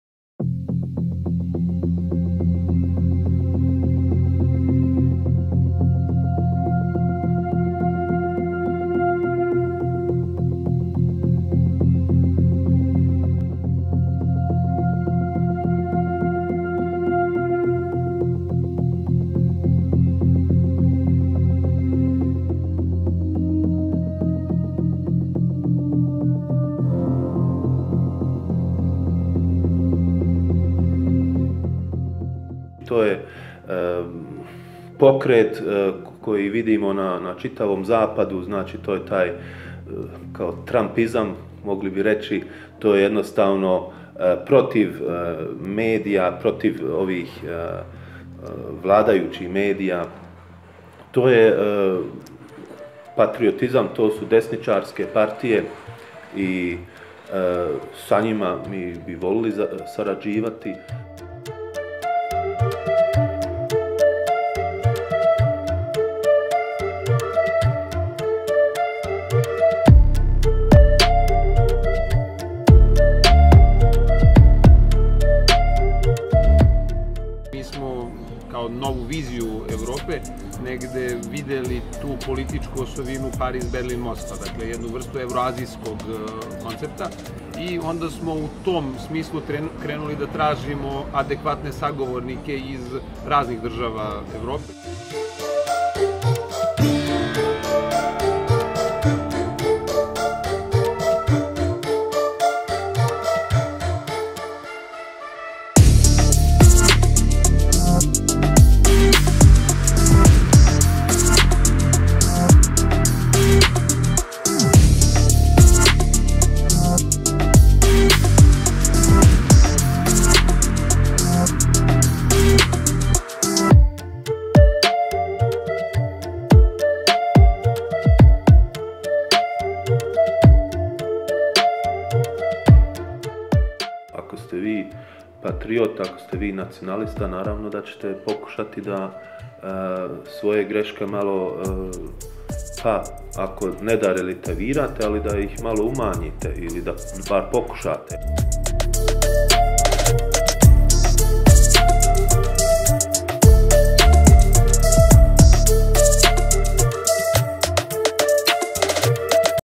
“We Are Their Voice”: German Far-Right Builds Balkan Alliances
For decades, the Balkans was seen as exporting instability to Western Europe. Now, Germany’s far-right is trying to extend its own ideology into southeastern Europe.